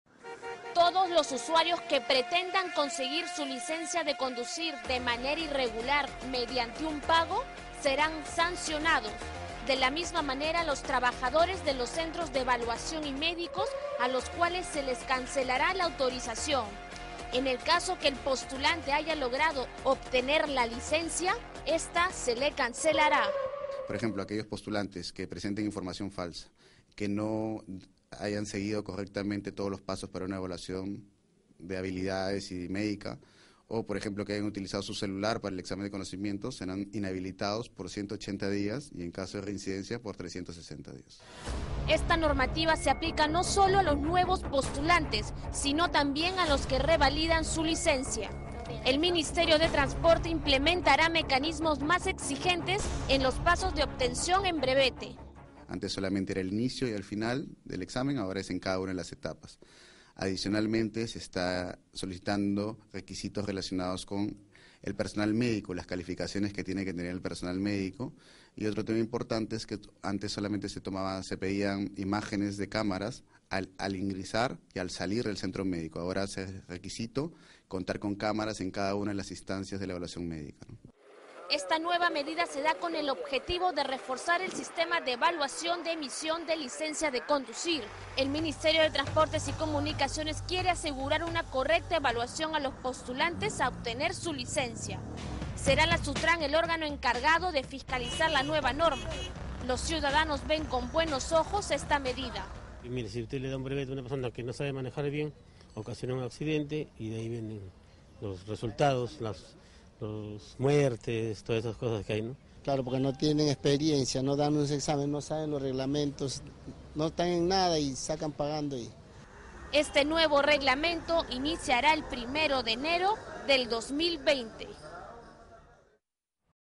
MTC sancionará a usuarios que obtengan de manera irregular su brevete
El Ministerio de Transporte y Comunicaciones (MTC) sancionará, a partir de la fecha, con la inhabilitación y cancelación de la licencia de conducir a aquellos postulantes que cometan actos irregulares para obtener dicho documento. El objetivo es fortalecer la seguridad vial en el país.

La disposición del MTC establece, por ejemplo, sanciones para los Centros encargados de la evaluación teórica y de manejo a los postulantes, que no cumplan de manera estricta con los procedimientos que la norma dispone.

Aquellos postulantes que presenten información falsa, que no hayan seguido correctamente pasos, o hayan utilizado su celular, serán inhabilitados por 180 días y en caso de reincidencia de 360 días", informó.